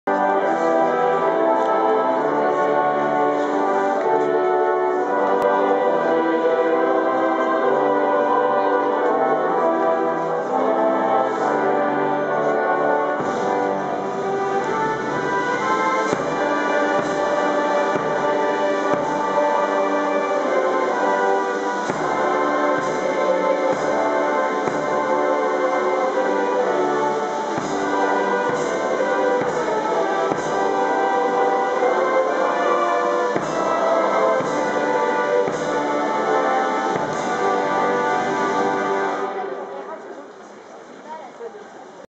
The Band of The Welsh Guards
VE Day Service of remembrance at the Cenotaph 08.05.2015